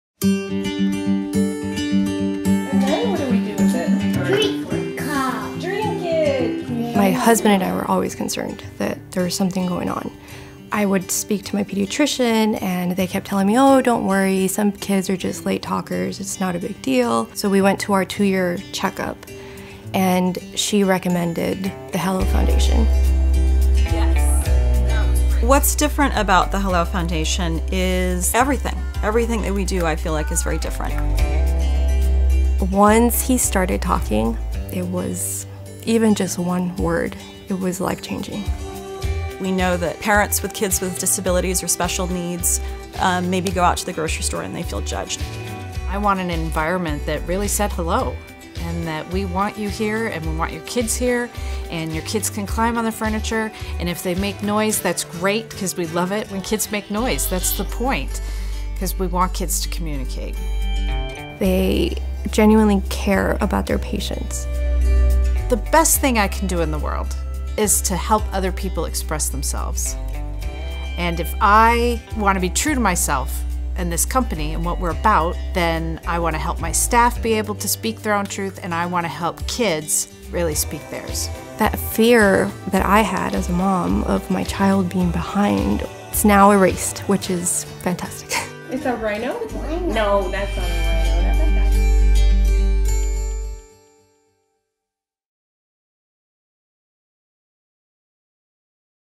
Speech Therapy with The Hello Foundation - Fear Can Take a Hike
We know that finding the right speech-language pathologist can be an emotional (and maybe even kinda scary) experience. But here's the thing: it doesn't have to be that way.